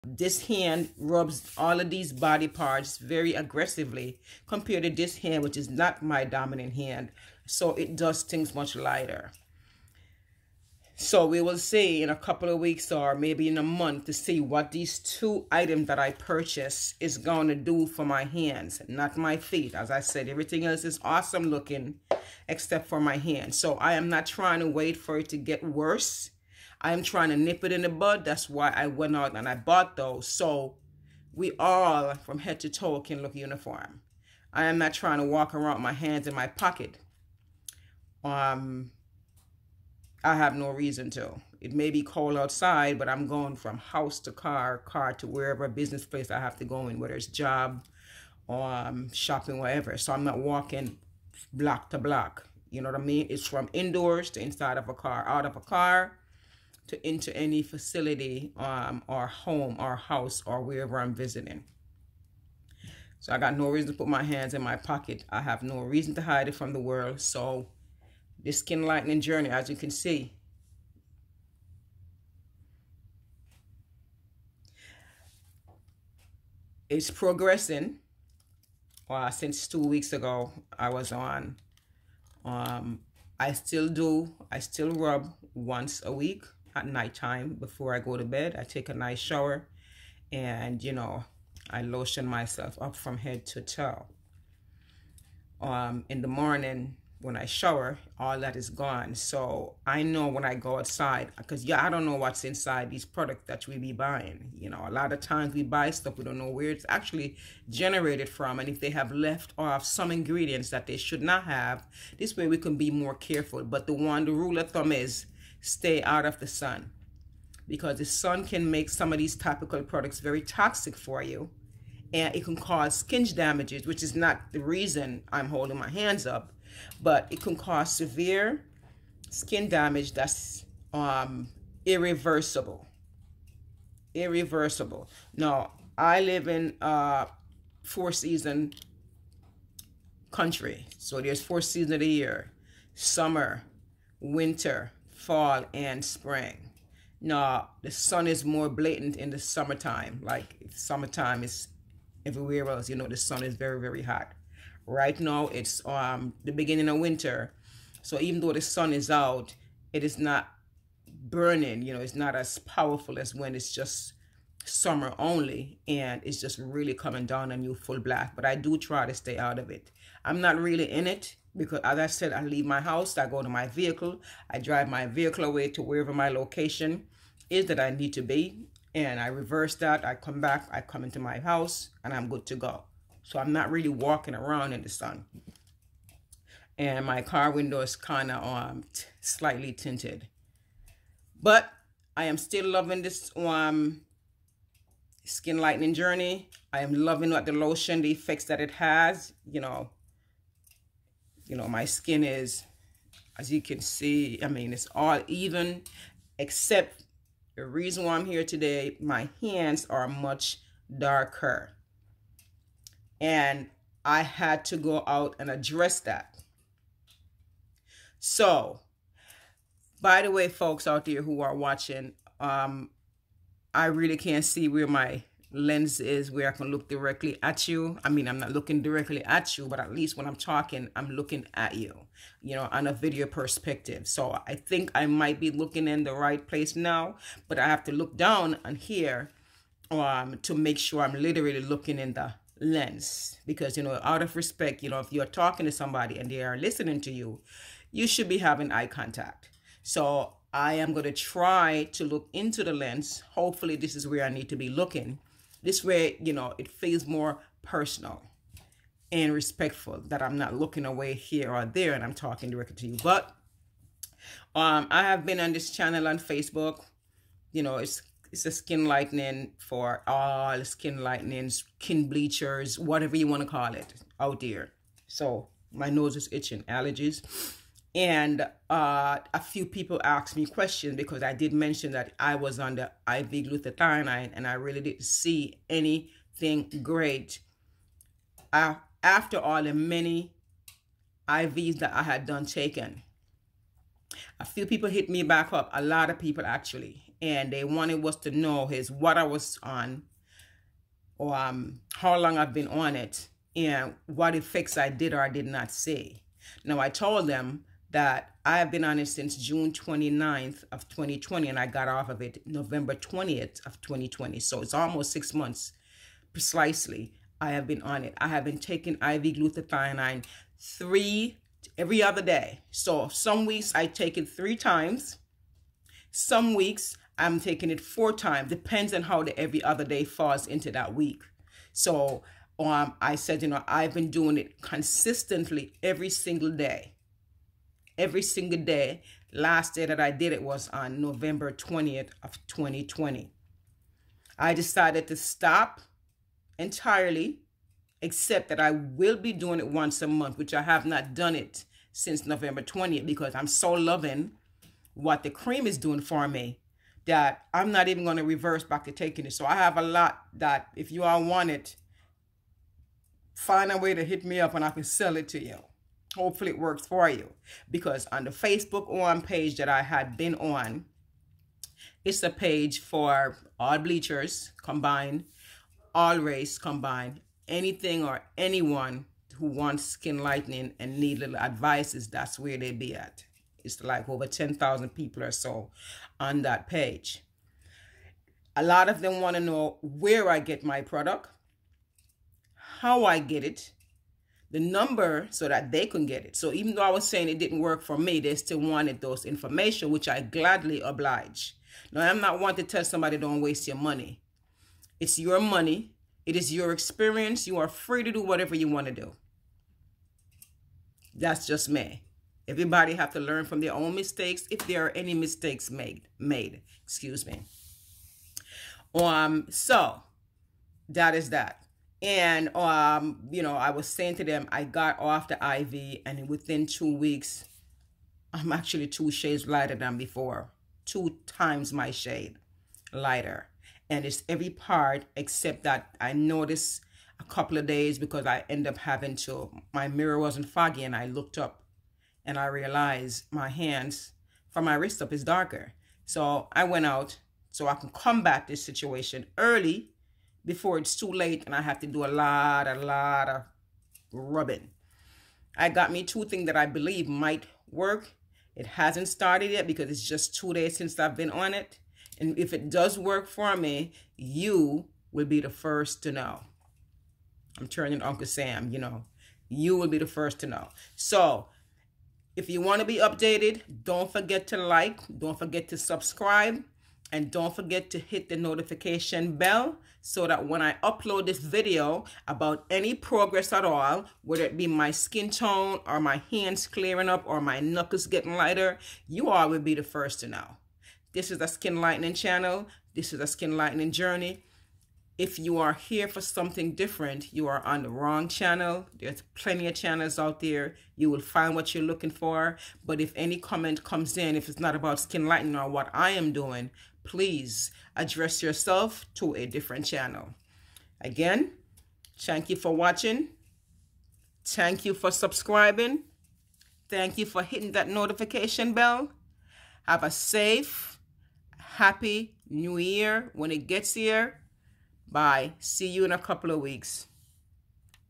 MY KNUCKLES/HAND IS GETTING DARKER.HOW TO LIGHTEN THEM BACK UP
SKIN WHITENING JOURNEY
I did made a mistake and said that I apply my products once a week it's a mistake.I apply my products once a day at night